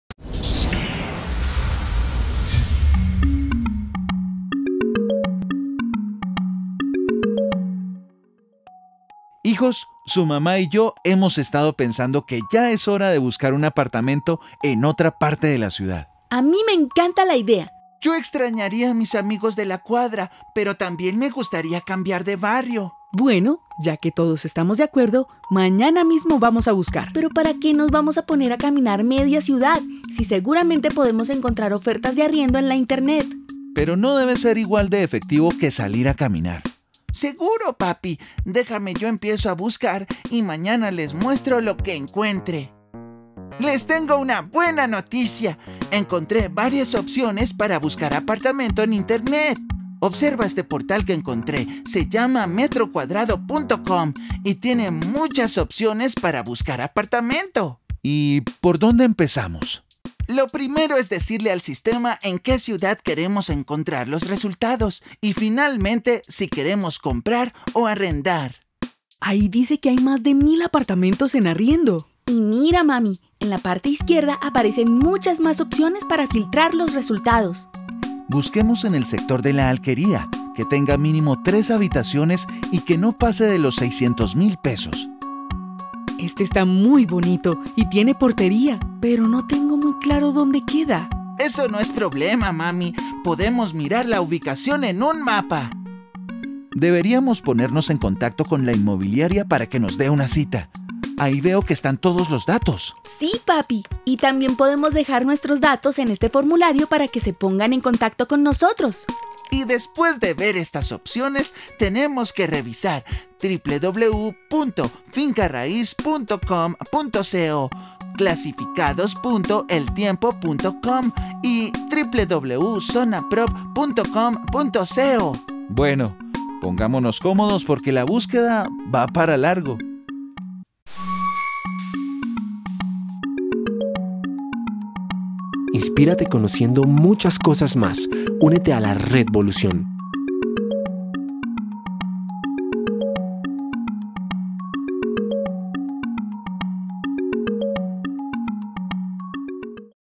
Cómo buscar vivienda en arriendo por internet
Conocer el procedimiento para buscar inmuebles en arriendo por internet.